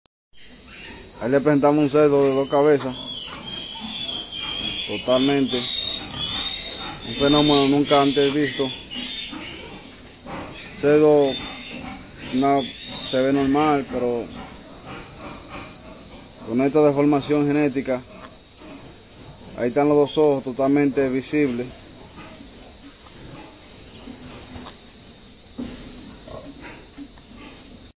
Rare two-headed pig born with four eyes and two snouts
A pig with two heads has been born in the central American country of Honduras.

The swine has two snouts and four eyes and both heads share the same body.

The reason for the two heads is a condition known as axial bifurcation.

This occurs when the embryo splits into two separate organisms but the process is incomplete, meaning the two never truly separate.